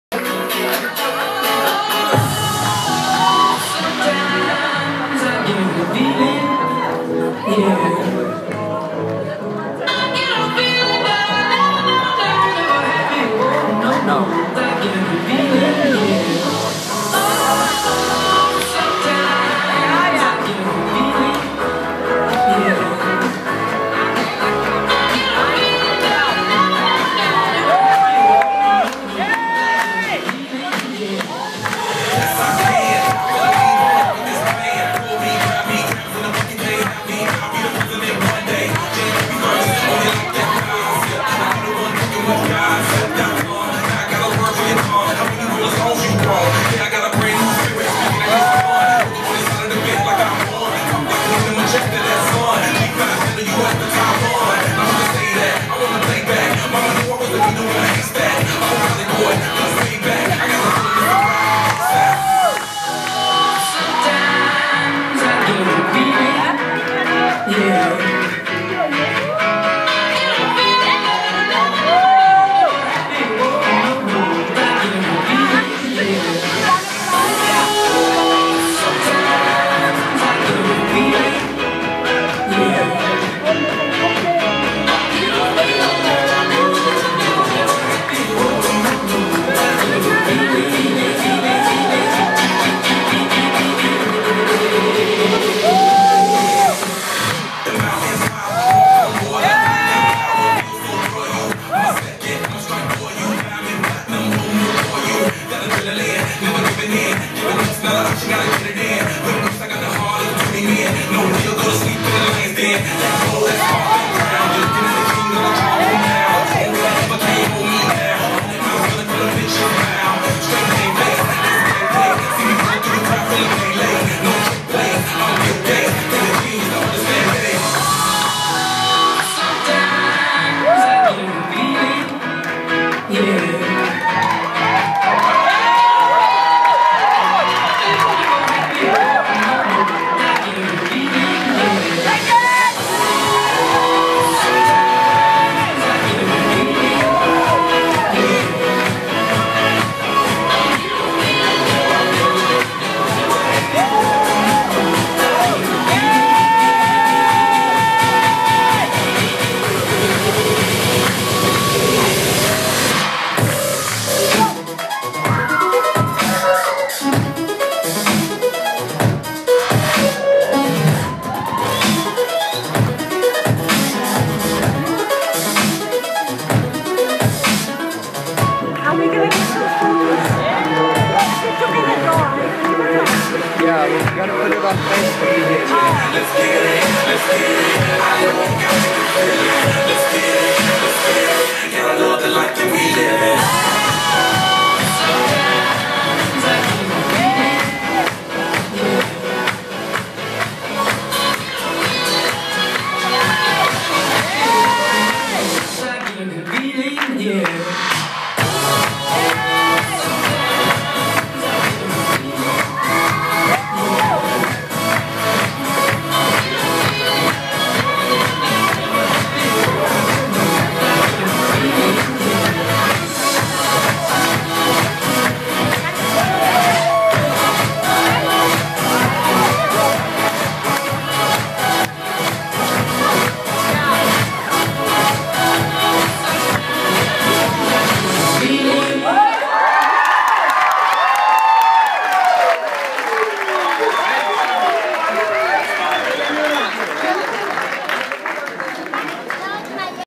The Brat Pack tear up the dance floor at Cantare Restaurant.
The Brat pack perform for parents and teachers of the Kim Field Dance Academy.